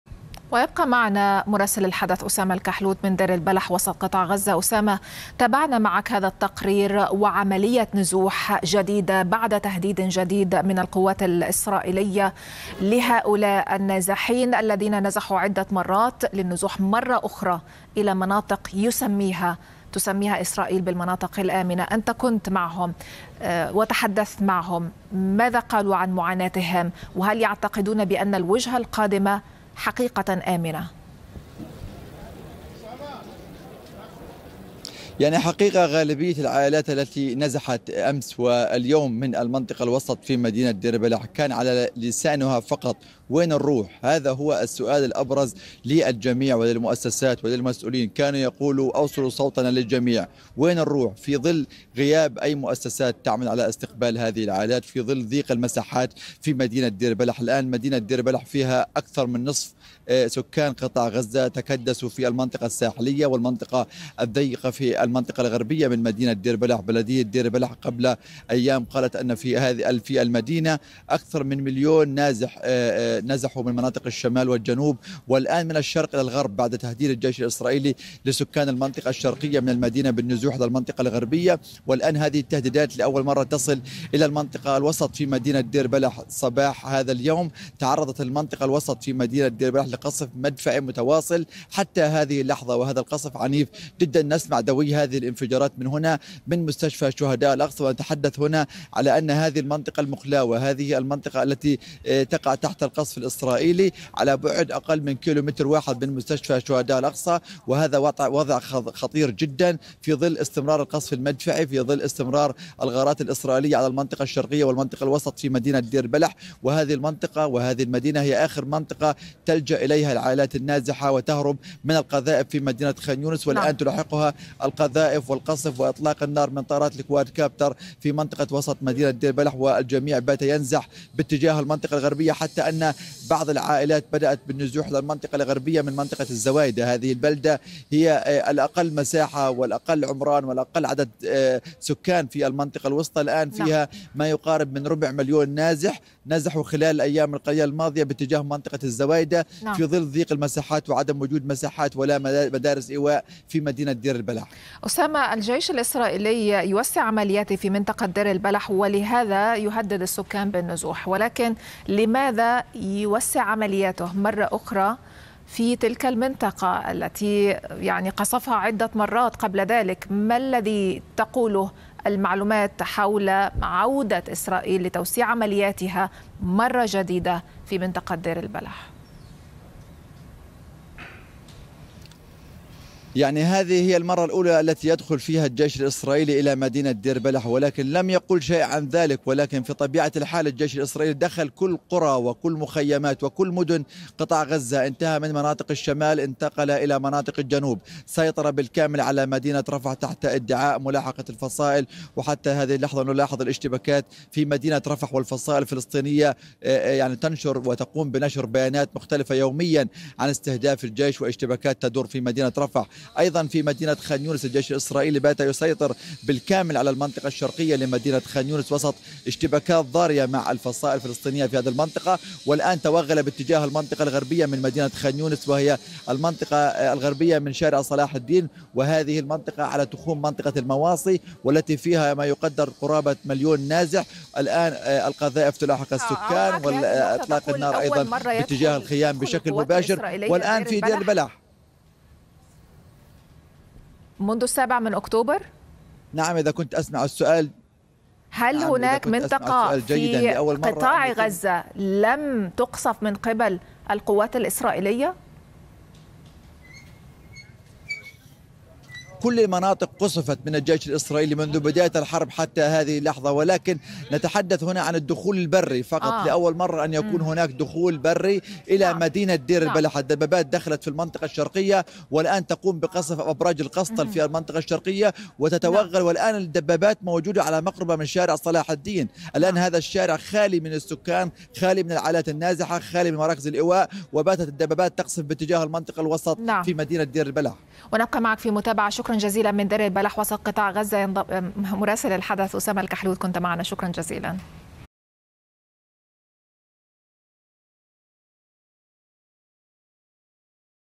هل هناك منطقة بغزة لم تقصف من إسرائيل؟ مراسل الحدث يكشف
هل هناك منطقة في غزة لم تقصف من الجيش الإسرائيلي؟ مراسل الحدث أسامة الكحلوت يجيب ويرصد تطورات الحرب على القطاع